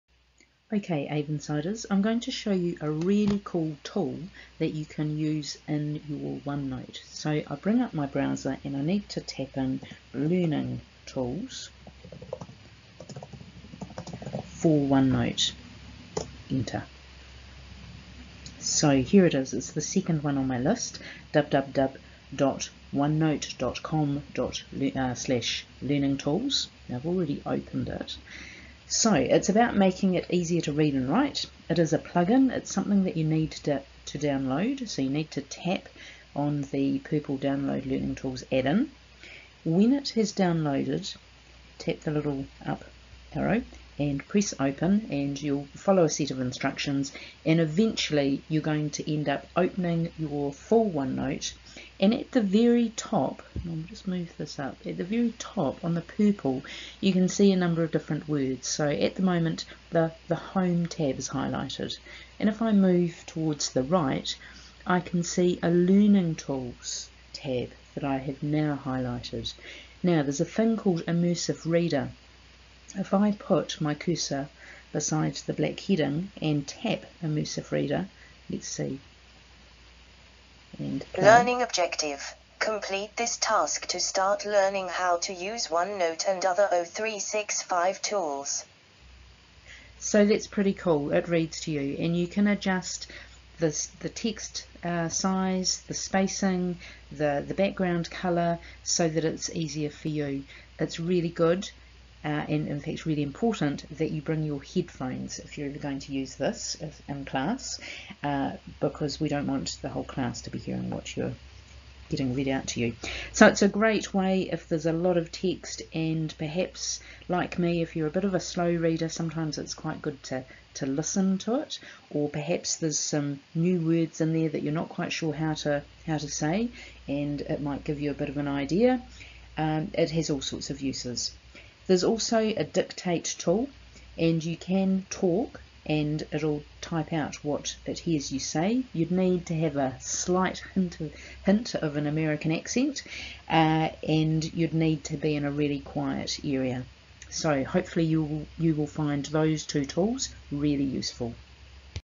OneNote Learning Tools
Learning Tools for OneNote, how to download and what they do.